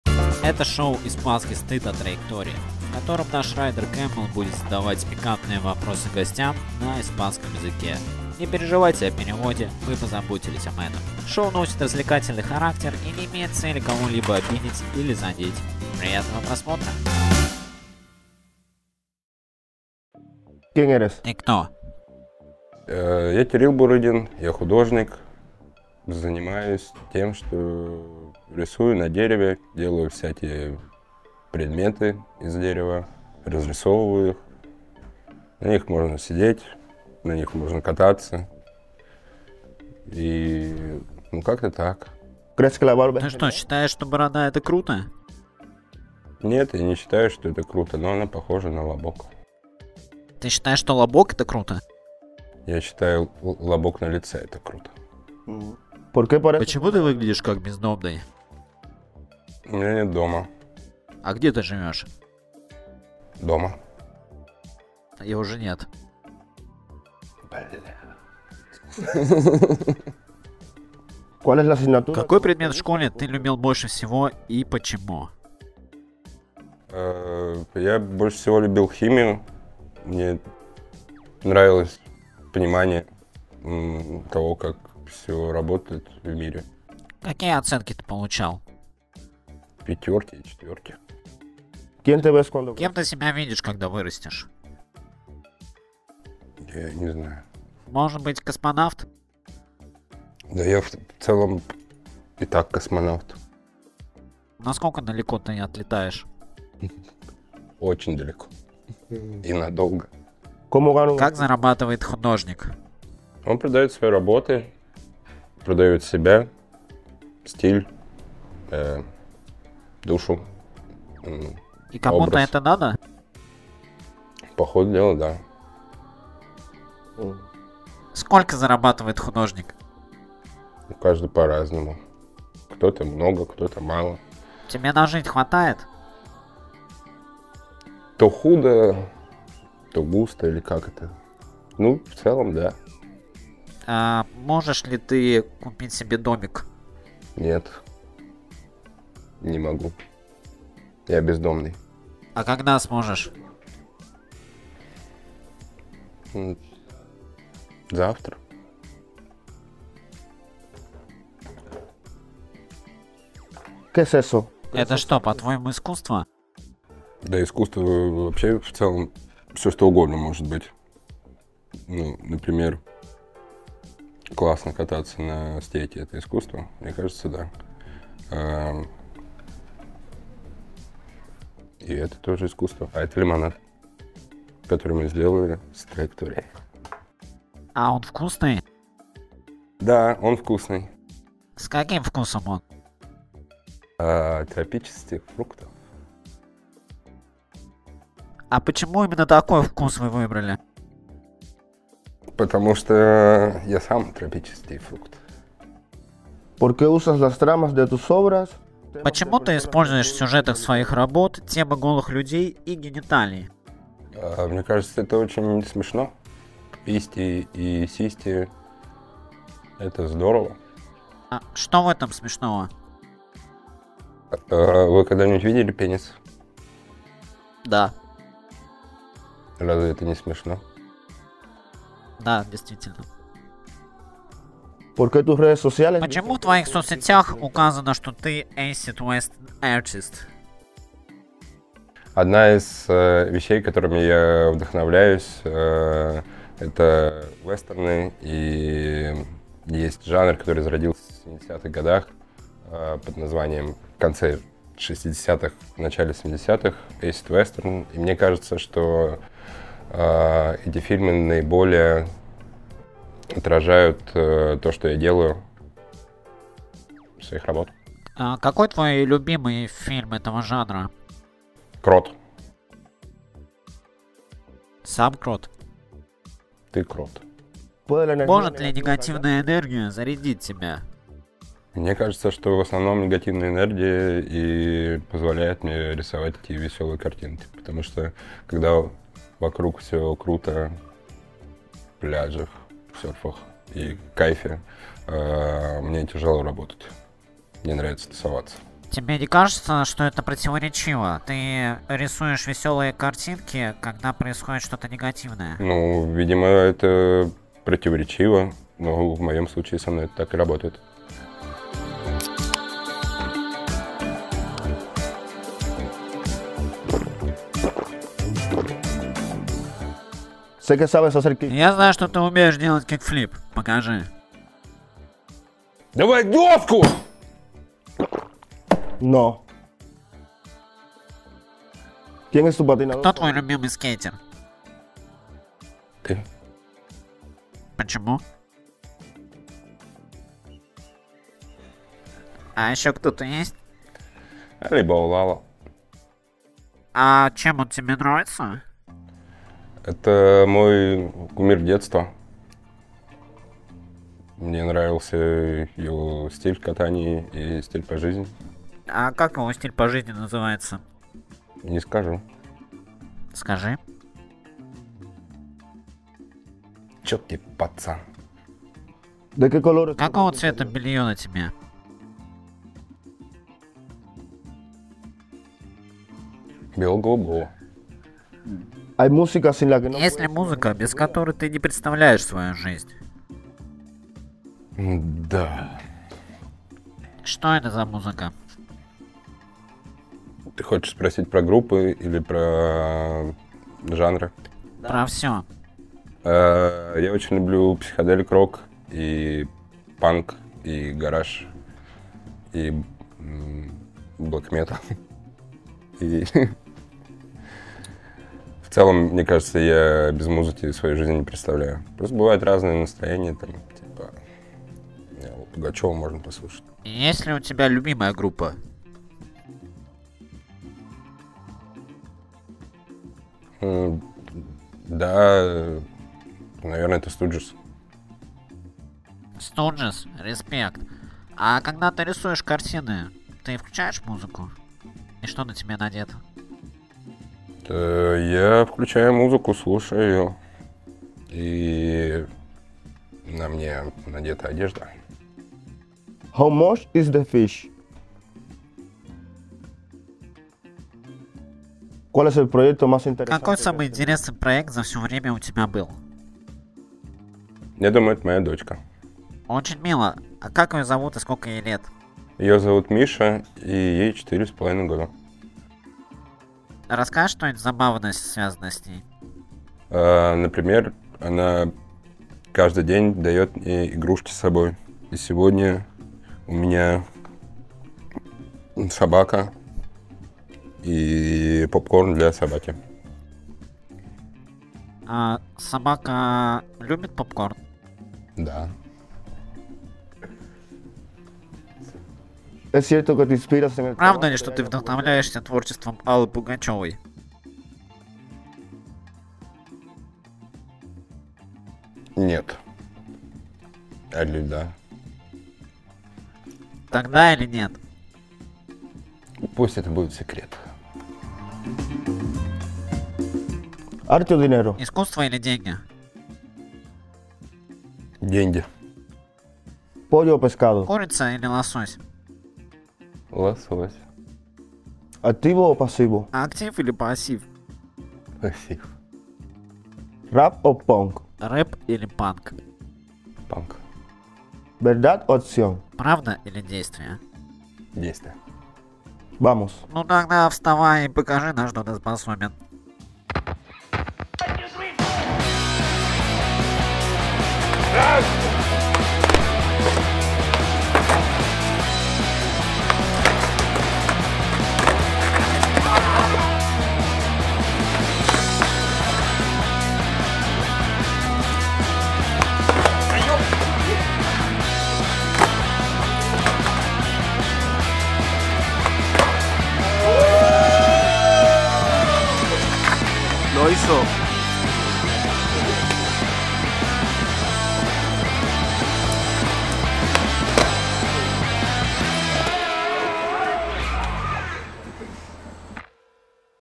Испанский стыд: Бурыгин - звезда или наплакано
Принимайте с распростертыми объятиями нашу новую серию видео «Испанский стыд», где мы задаем неприличные вопросы приличным гостям.

Наша первая звездочка - московский художник Кирилл Бурыгин, чей примитивистский живописный стиль вдохновлен элементами массовой и народной культуры.

Локация: Тебураси (Страстной бул., 7, стр. 3)